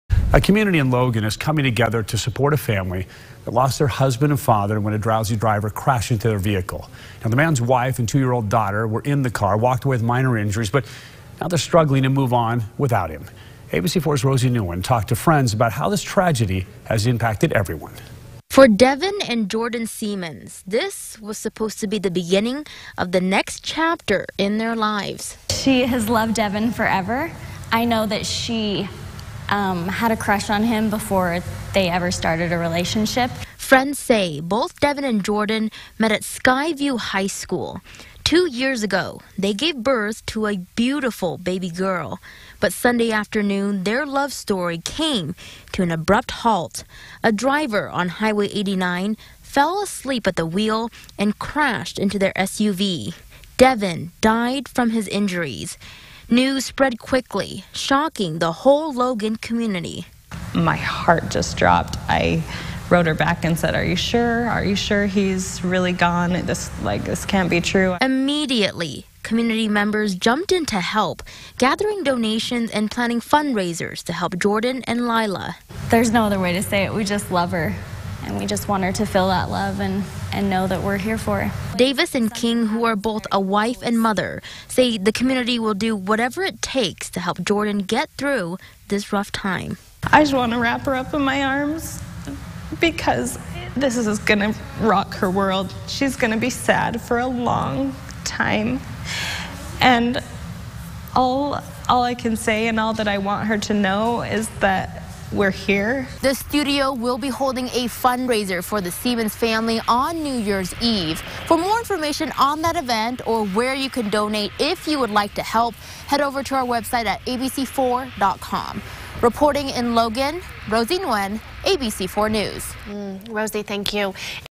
Community rallies behind family of Logan man killed in drowsy driving crash (5 p.m.)
Community members are rallying behind the family of a Logan man who was killed in a drowsy driving crash Sunday.